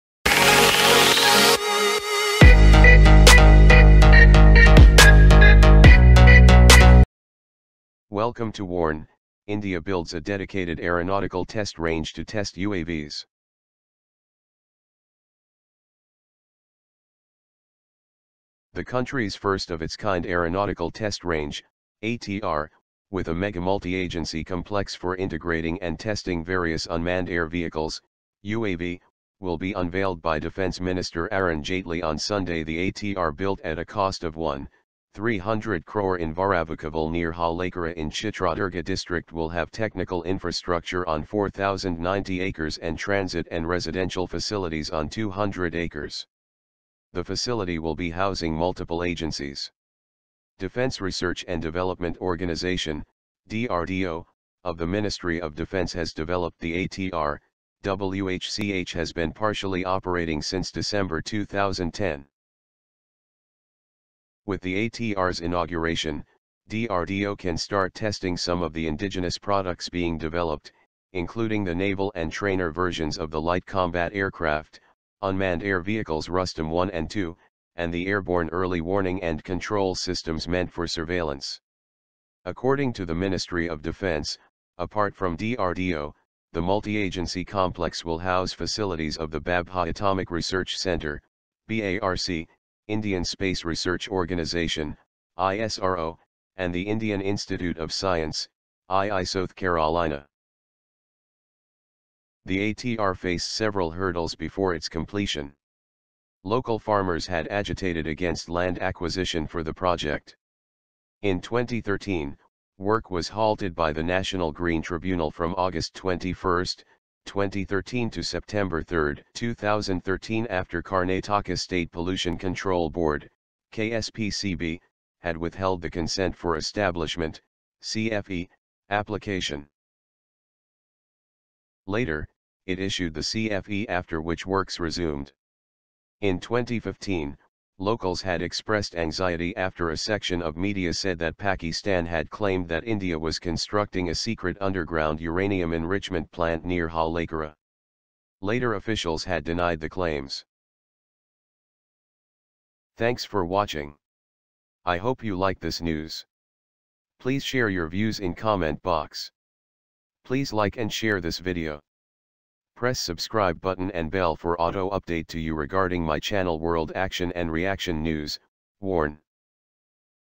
India builds a dedicated Aeronautical Test Range to test UAVs
The country’s first of its kind Aeronautical Test Range (ATR) with a mega multi-agency complex for integrating and testing various Unmanned Air Vehicles (UAV) will be unveiled by Defence minister Arun Jaitley on Sunday.The ATR built at a cost of `1,300 crore in Varavoo Kaval near Challakere in Chitradurga district will have technical infrastructure on 4,090 acres and transit and residential facilities on 200 acres. The facility will be housing multiple agencies .

Defence Research and Development Organisation (DRDO) of the Ministry of Defence has developed the ATR, whch has been partially operating since December 2010.

With the ATR’s inauguration, DRDO can start testing some of the indigenous products being developed, including the naval and trainer versions of the Light Combat Aircraft, unmanned air vehicles Rustom 1 and 2; and the Airborne Early Warning & Control Systems meant for surveillance.

According to the Ministry of Defence, apart from DRDO, the multi-agency complex will house facilities of the Bhabha Atomic Research Centre (BARC), Indian Space Research Organisation (ISRO) and the Indian Institute of Science (IISc).

The ATR faced several hurdles before its completion. Local farmers had agitated against land acquisition for the project.

In 2013, work was halted by the National Green Tribunal from 21 August, 2013 to 03 September 2013 after Karnataka State Pollution Control Board (KSPCB) had withheld the ‘Consent for Establishment’ (CFE) application.

Later, it issued the CFE after which works resumed.

In 2015, locals had expressed anxiety after a section of media said that Pakistan had claimed that India was constructing a secret underground Uranium Enrichment Plant near Challakere. Later officials had denied the claims.